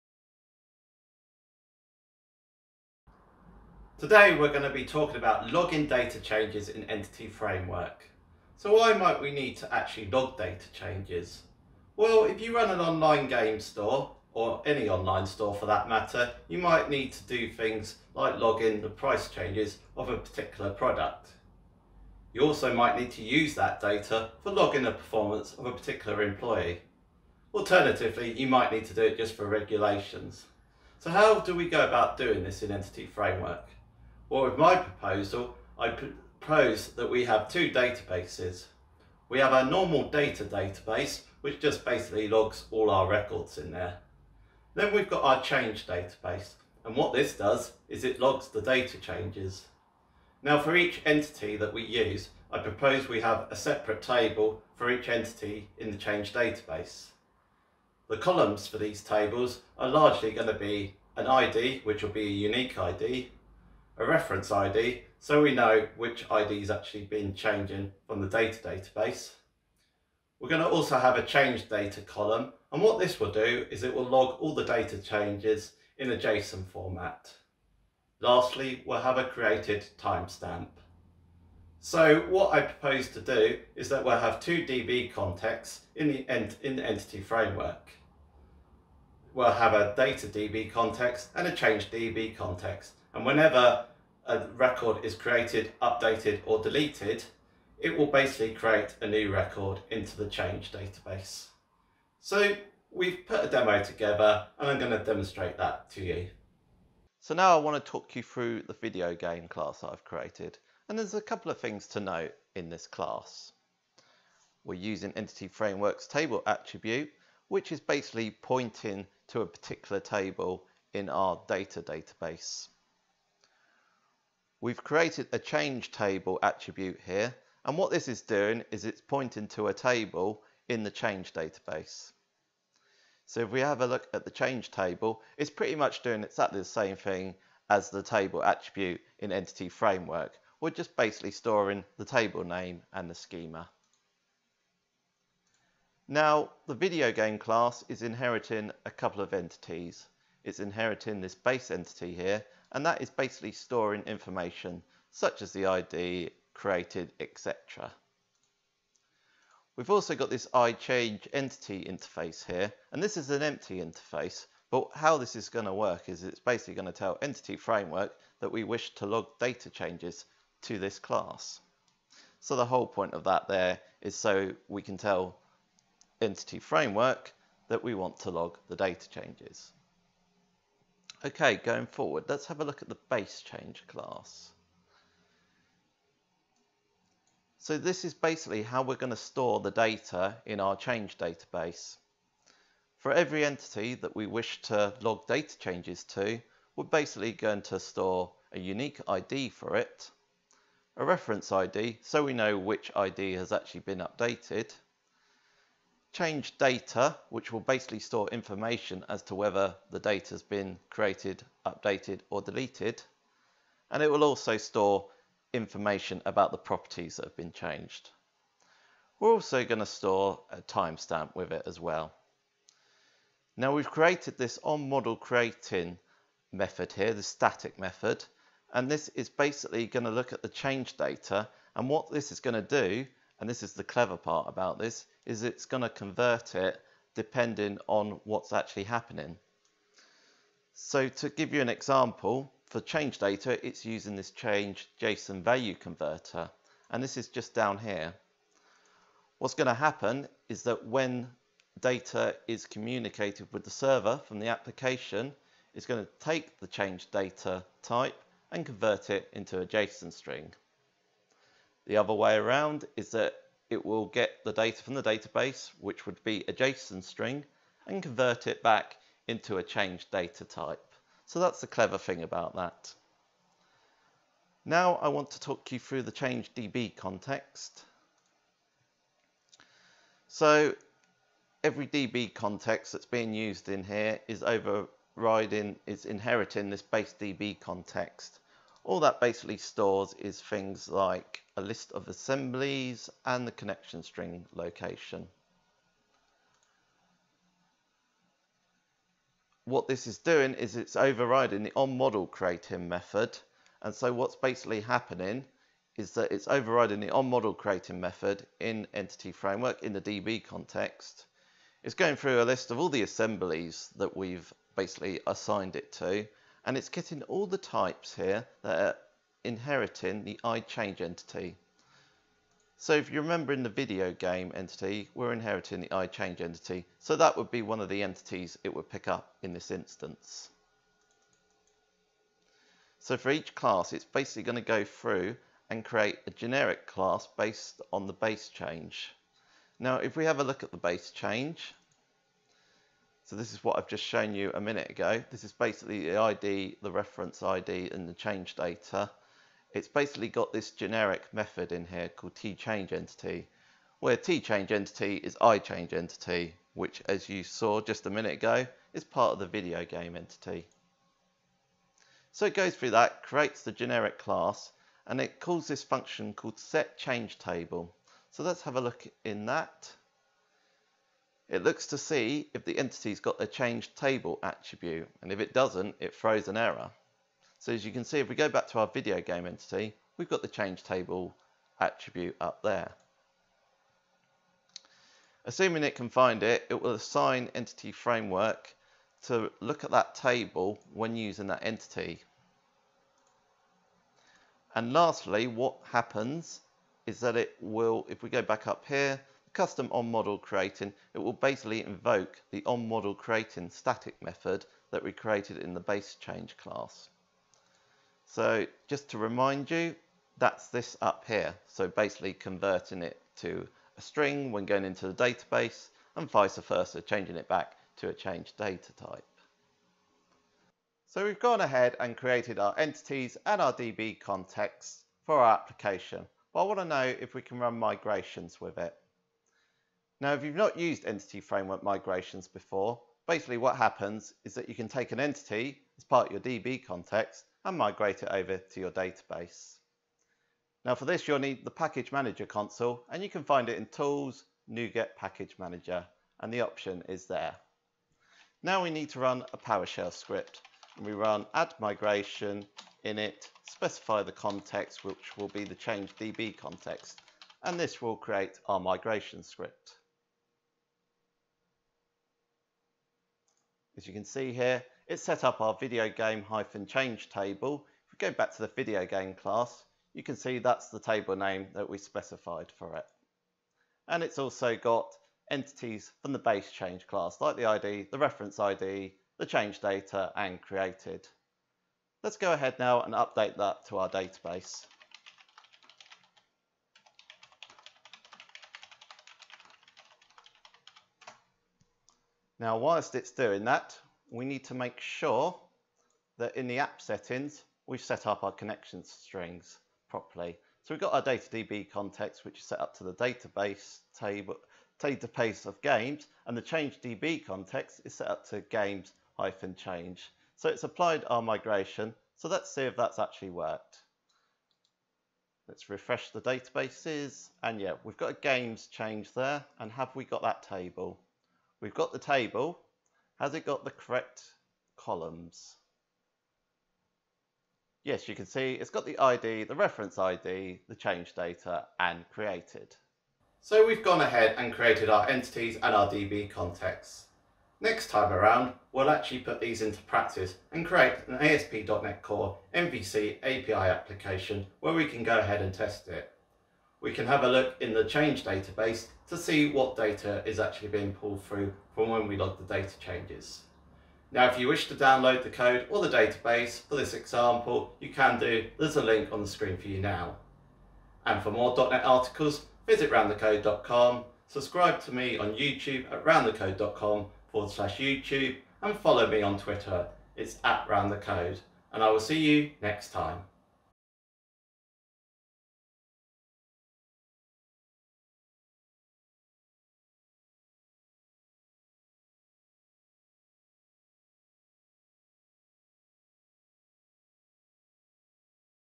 Log Data Changes in Entity Framework - Part 1 - Create Entities and DbContext's
It's become more important to be able to log data changes in Entity Framework, but how do you go about and do that?

In this example, we will have two databases, with each database having it's own DbContext. Our "data" database will store our records for our entities. The other database will be our "change" database. This will have a separate table for each entity that we wish to log data changes to. However, the columns for these tables will be the same.

We will go ahead and set up our entities. Afterwards, we will set up our DbContext's and integrate them with Entity Framework. Finally, we will use EF Migrations for both our DbContext's to create our databases, and create the relevant tables.